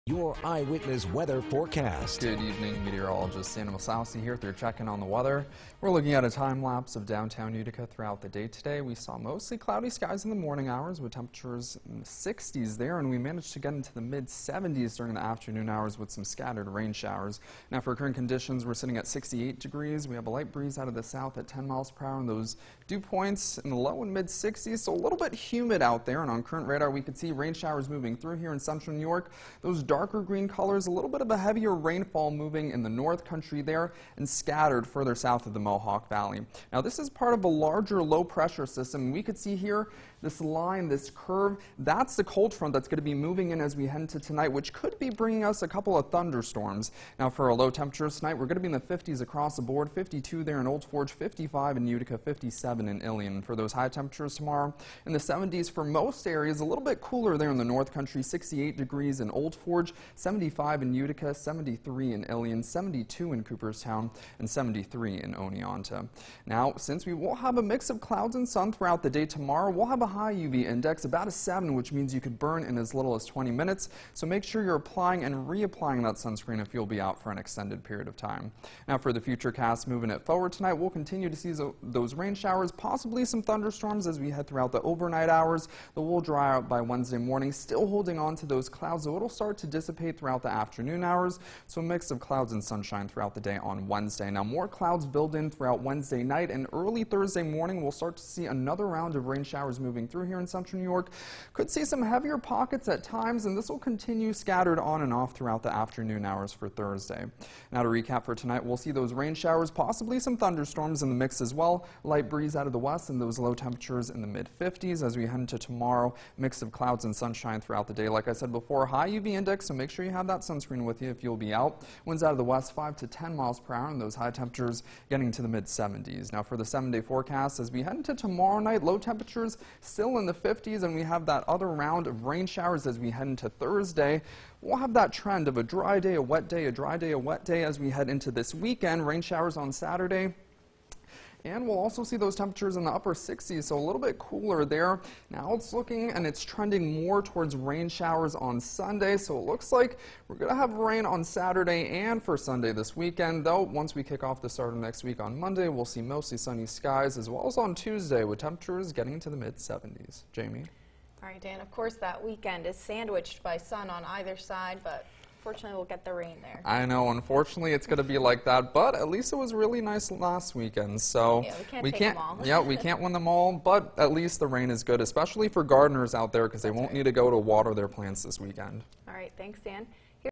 Eyewitness Weather Forecast from 6/7/2022
Meteorologist Daniel Maslowski with your Eyewitness Weather Forecast from 6/7/2022